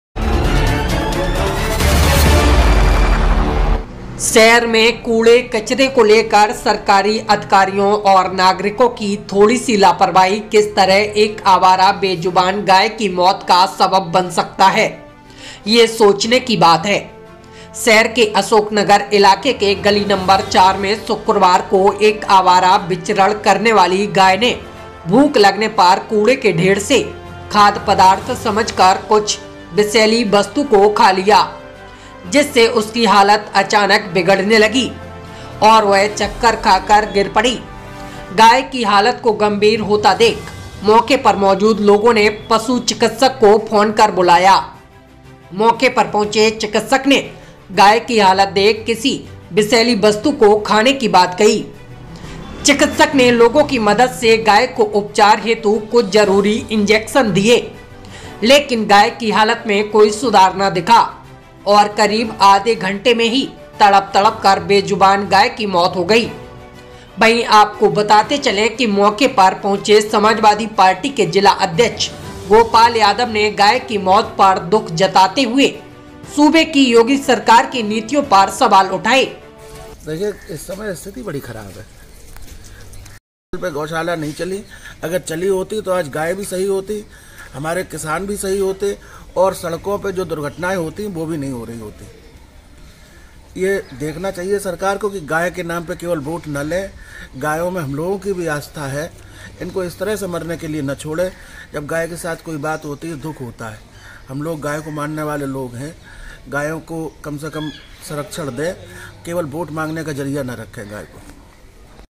जहर खिलाकर गाय को मारने वाला कौन है ? - Etawah
MSS न्यूज़

रिपोर्ट- शिवांग तिमोरी

इटावा यूपी

शहर में कूड़े-कचरे को लेकर सरकारी अधिकारियों और नागरिकों की थोड़ी सी लापरवाही किस तरह एक आवारा बेजुबान गाय की मौत का सबब बन सकता है ये सोचने की बात है. शहर के अशोक नगर इलाके के गली नंबर 4 में शुक्रवार को एक आवारा विचरण करने वाली गाय ने भूख लगने पर कूड़े के ढ़ेर से खाघ पदार्थ समझकर कुछ विषाक्त वस्तु को खा लिया जिससे उसकी हालात अचानक बिगड़ने लगी और वह चक्कर खाकर गिर पड़ी. गाय की हालत को गंभीर होते देख मौके पर मौजूद लोगों ने पशु चिकत्सक को फोन कर बुलाया. मौके पर पहुंचे चिकत्सक ने गाय की हालत देख किसी विषाक्त वस्तु को खाने की बात कही. चिकत्सक ने लोगों की मदद से गाय को उपचार हेतु कुछ जरूरी इंजेक्शन दिए लेकिन गाय की हालात में कोई सुधार न दिखा और करीब आधे घंटे में ही तड़प-तड़प कर बेजुवान गाय की मौत हो गई.

वहीं मौके पर पहुंचे समाजवादी पार्टी के जिला अध्यक्ष गोपाल यादव ने गाय की मौत पर दुख जताते हुए सूबे की योगी सरकार की नीतियों पर सवाल उठाए. गोपाल यादव ने कहा कि सरकार अगर चिंतित होती तो आज प्रदेश में आवारों गायों और गोवंशों की ये हालत न होती. उन्होंने कहा कि योगी सरकार की गोशालाएं अगर वास्तव में होती तो आवारा गायों और गोवंश के द्वारा सूबे के किसानों की न तो फसलें बर्बाद होतीं न सड़कों पर दुर्घटनाएं होतीं और न हीं गायों की ये हालत होती. सपा अध्यक्ष ने दुख जताते हुए कहा कि हमारी भी गायों में आस्था है योगी सरकार गायों के नाम पर केवल वोट न ले, सरकार आवारा जानवरों को उचित संरक्षण दें उन्हें केवल वोट मांगने का जरिया न रखे.

वहीं गाय की इस तरह असमय मौत पर गली के लोगों ने शोक जताया. मौके पर उपस्थित लोगों ने नगरपालिका की मदद से गाय के मृत शरीर को निस्तारण के लिए भेज दिया. एक जिम्मेदार नागरिक होने के नाते ये हर एक व्यक्ति की जिम्मेदारी है कि हम इस बात को जिम्मेदारी लें कि हमारे घरों से निकलने वाले खाघ पदार्थों को पॉलीथिन आदि में भर कर न फैंका जाए. जिससे अनजाने में ही सही लेकिन हम किसी बेजुवान जानवर की मौत के कारण न बनें.

mssnews.site123.me